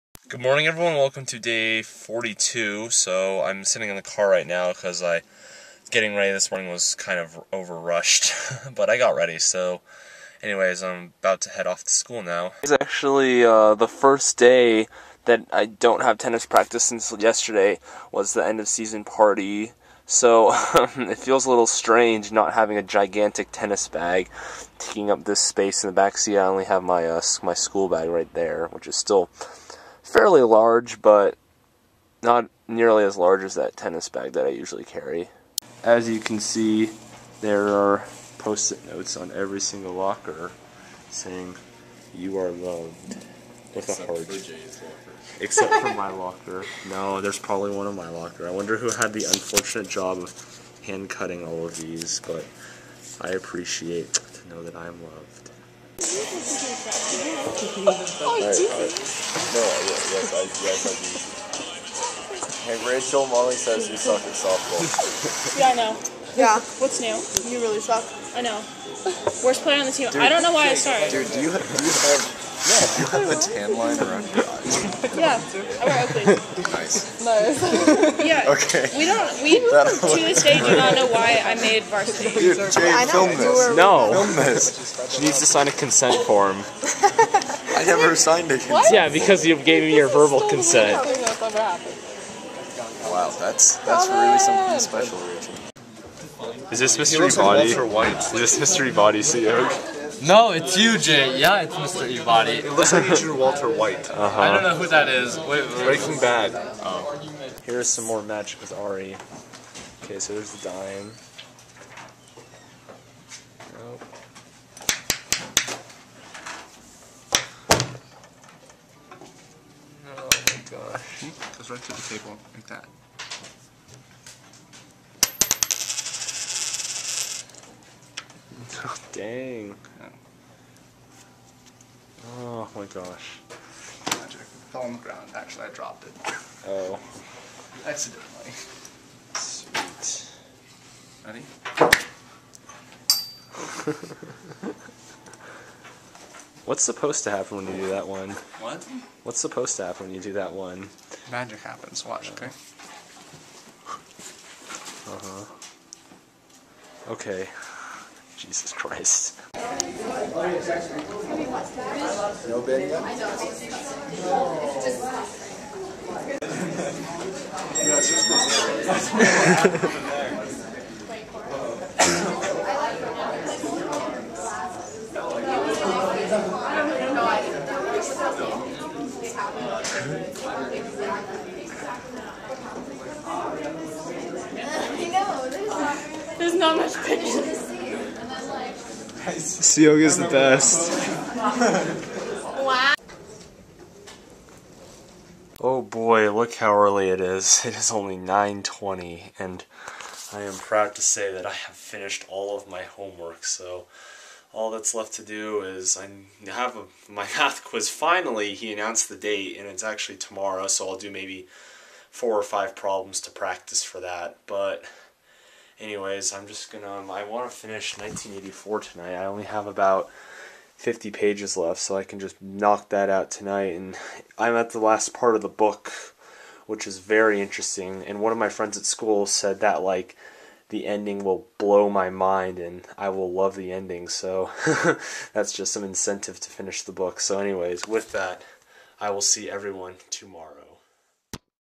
Teacher Caricatures - (Day 42 - September 30th, 2014)
Today was my first day that I drove to school without a heavy tennis bag in my car. Suyog draws some funny pictures of our teachers and Ari does some more magic.